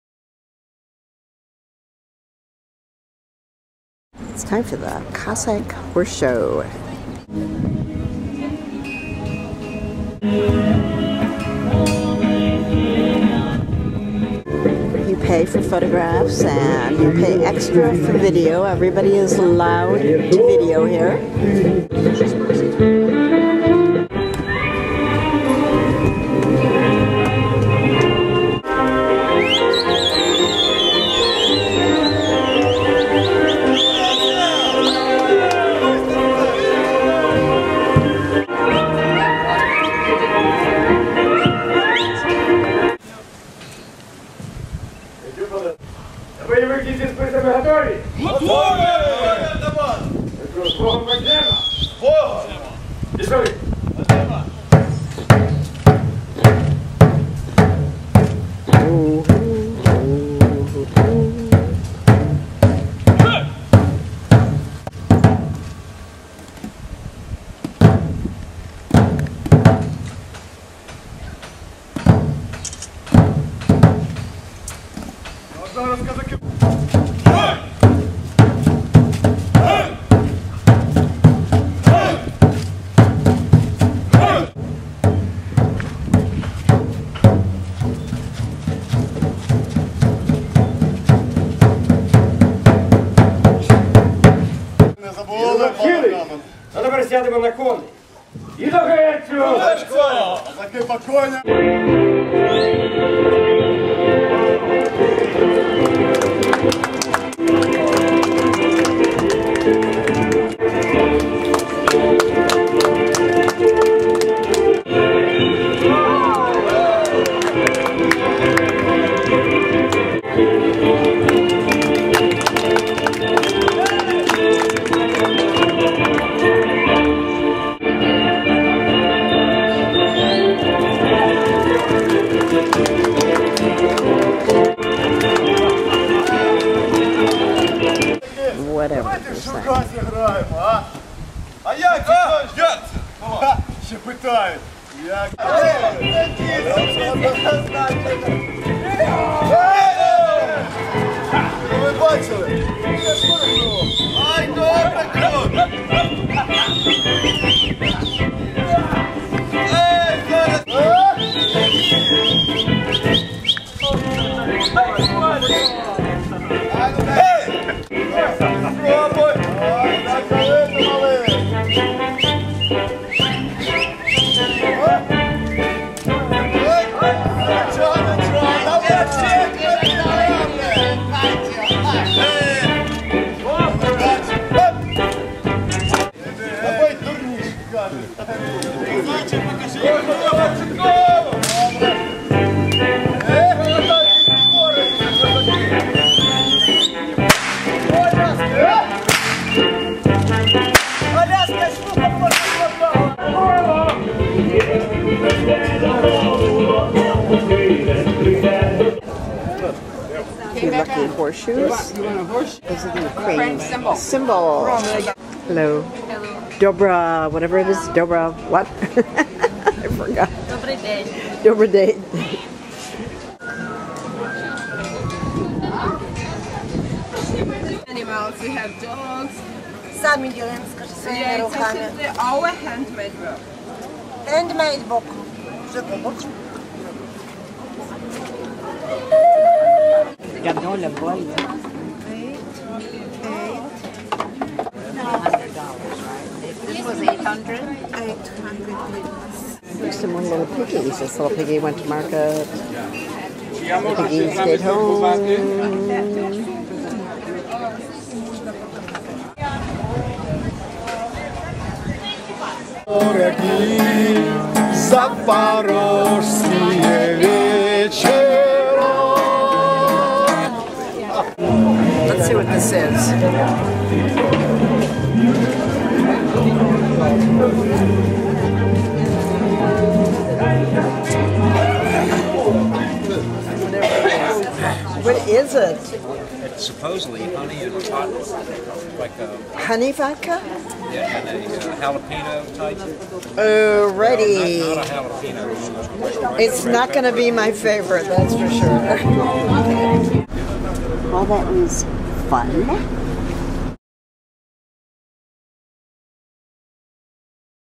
Viking Cruise Lomonosov: A Cossack Horsemanship Show Zaporozhye, Ukraine
The Khortitsa Cossack Show was enjoyable and worth sitting through (even though memories of stunt shows at Disney World ran through my mind). Men performed wonderful feats of horsemanship. There were songs and the pseudo-Cossack village was entertaining.

Samples of local honey-peppery-vodka was handed to all passengers along with a plate of typical Pilaf. Pilaf? Thumbs up. Vodka? Gag...choke...awful...